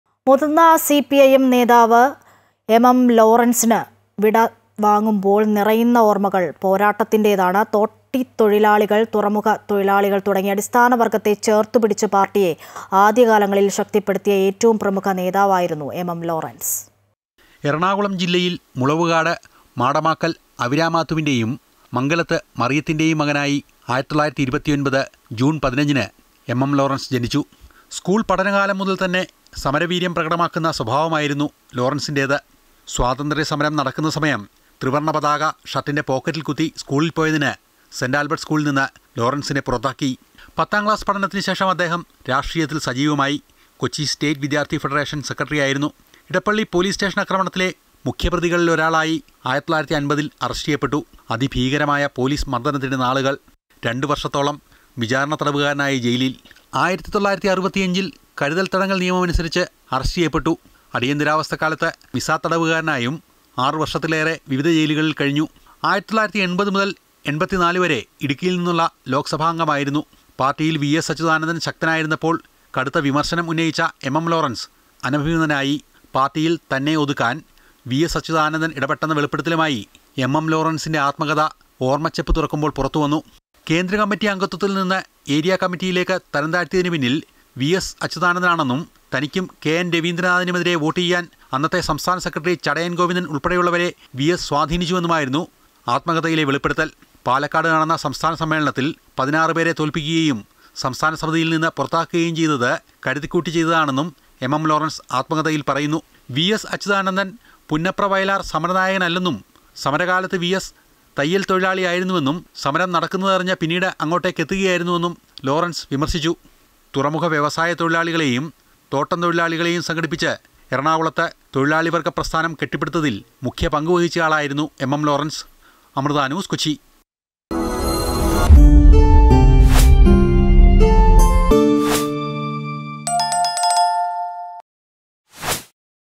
എം എം ലോറൻസിന്‍റേത് പോരാട്ടങ്ങൾ നിറഞ്ഞ ഓർമ്മകൾ | Amrita News
മുതിർന്ന സിപിഐഎം നേതാവ് എം എം ലോറൻസ് വിട വാങ്ങുമ്പോൾ നിറയുന്ന ഓർമ്മകൾ പോരാട്ടത്തിൻ്റേതാണ്. തോട്ടിത്തൊഴിലാളികൾ, തുറമുഖത്തൊഴിലാളികൾ തുടങ്ങി അടിസ്ഥാനവർഗ്ഗത്തെ ചേർത്തുപിടിച്ച് പാർട്ടിയെ ആദ്യകാലങ്ങളിൽ ശക്തിപ്പെടുത്തിയ ഏറ്റവും പ്രമുഖ നേതാവായിരുന്നു എം എം ലോറൻസ്.